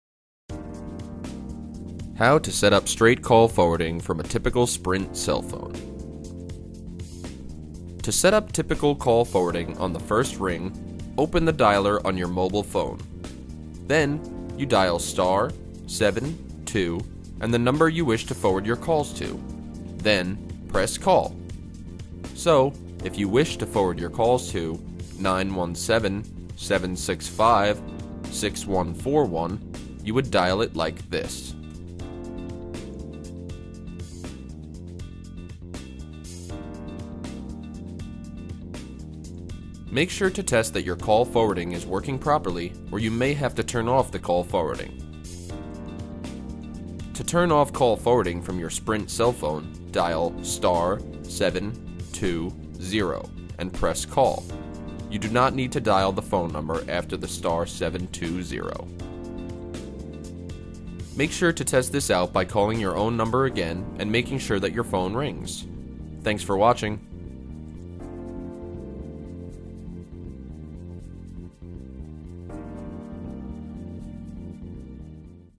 Sprint - How To Set Up Straight Call Forwarding
How To Set Up Straight Call Forwarding From Sprint Cell Phone.